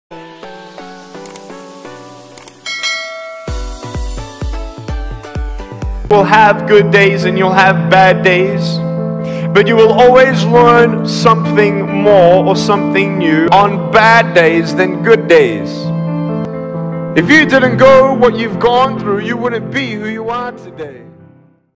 Stay Positive, learn from your bad days
Motivational video from Kuya_Bertlog MotVid collection

Speaker:
Nick Vujicic